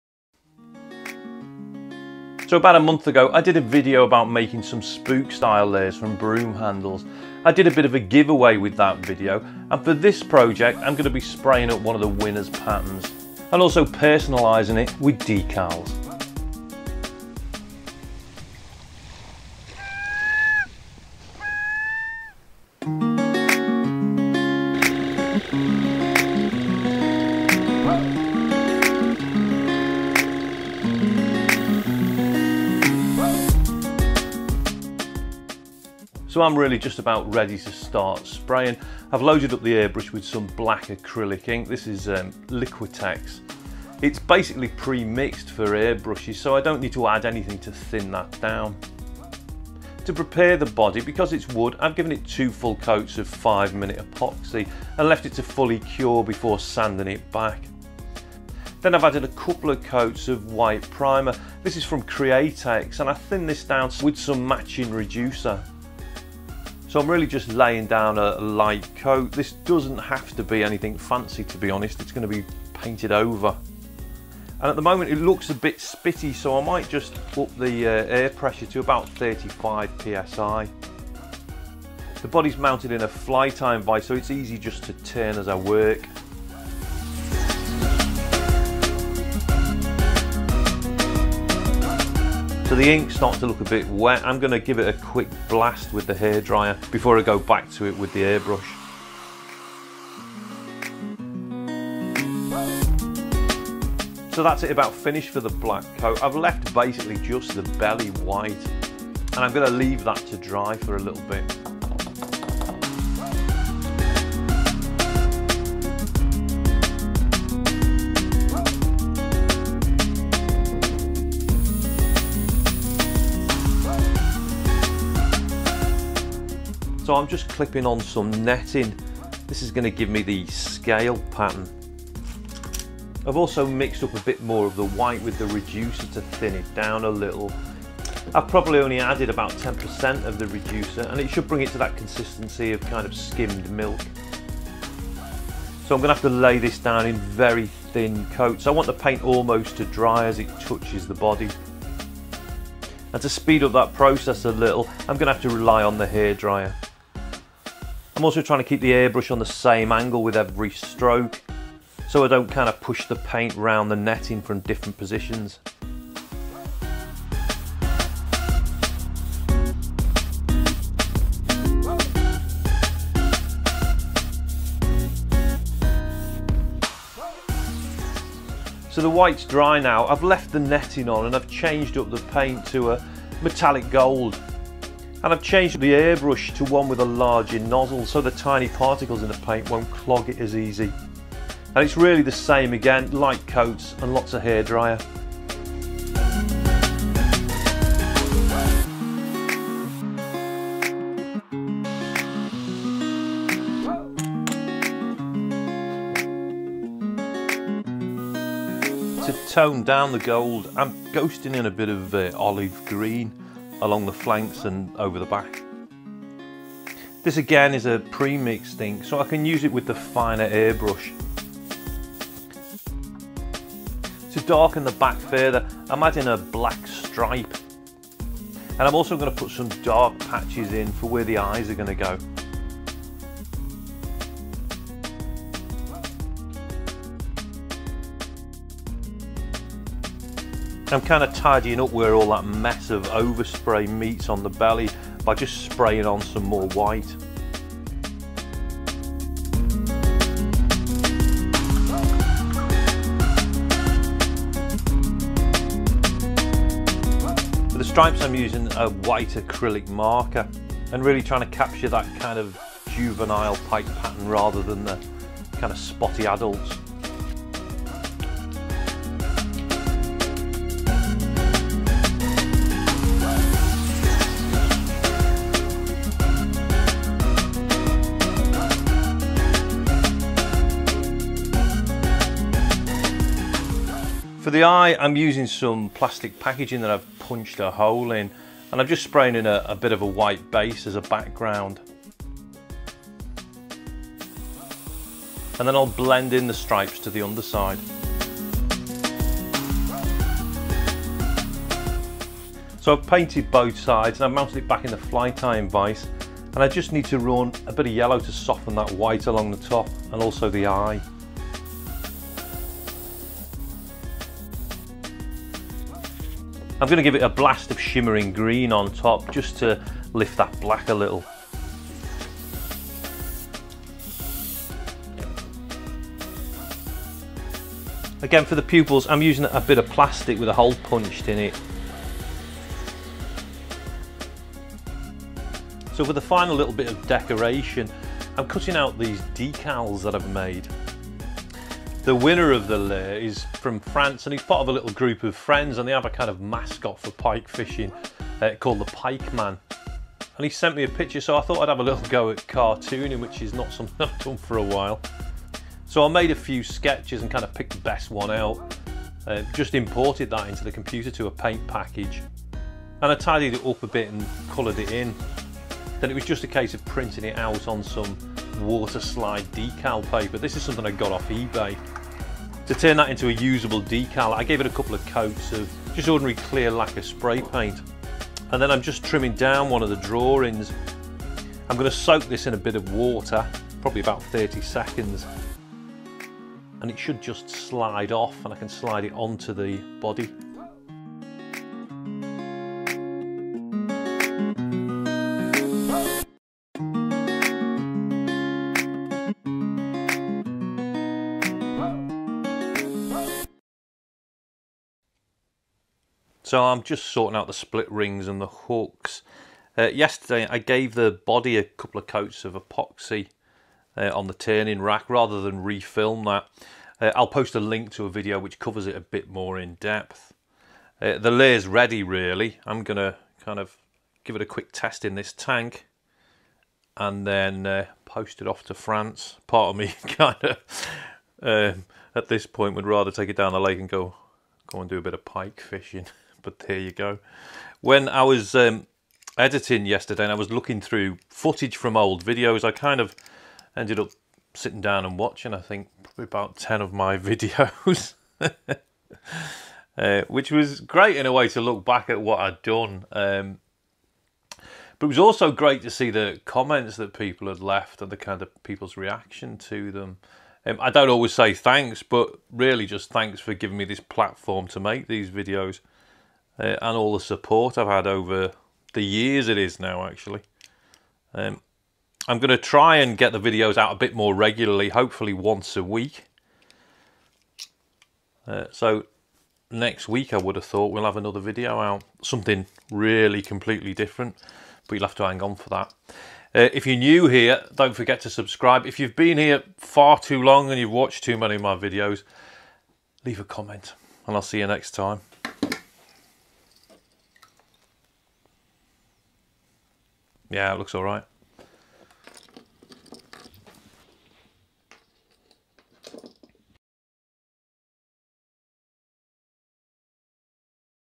Lure painting a basic pike pattern and using decals
Fantastic Video By Paul Adams "The Handmade Fisherman" giving you a step by step on how to airbrush a pike fishing lure with decals.

for all your airbrushing needs.